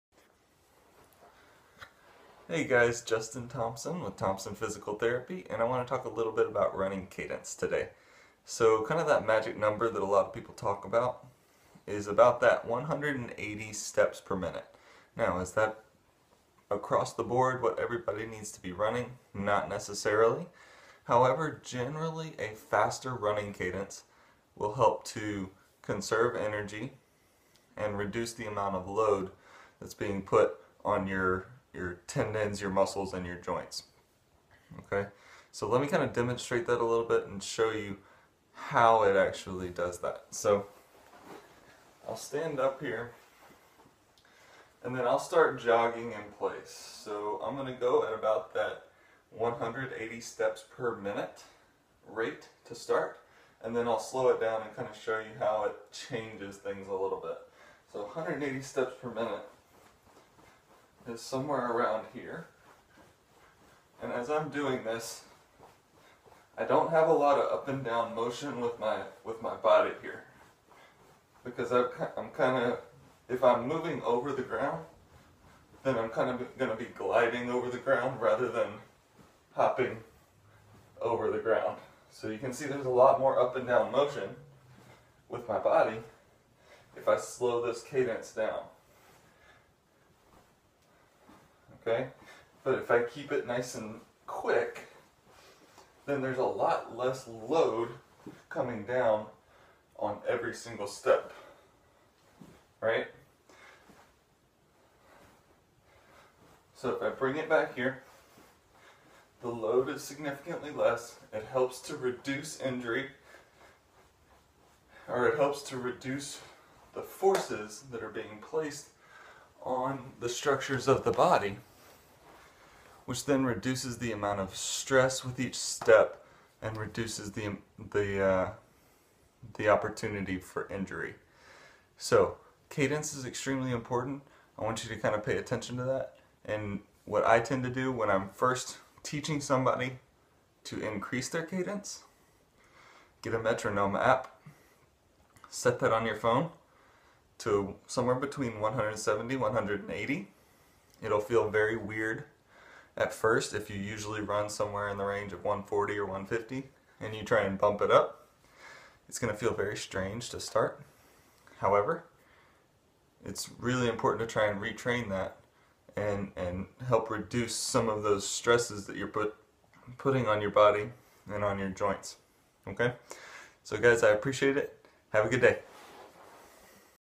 Increase Running Cadence To Reduce Injury
A quick demonstration of how your running cadence can affect the load that is being placed on your muscles, tendons, ligaments, and joints.